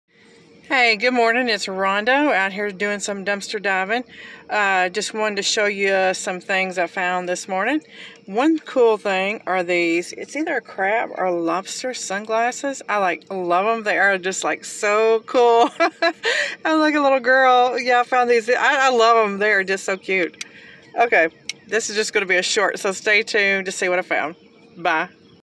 DUMPSTER DIVING Too Cool Sunglasses
Hello everyone. I found these cool shades in the dumpster and they didn't break them. I'm so happy!!! Thanks to everyone who subscribes, likes and comments! Y'all are THE BEST!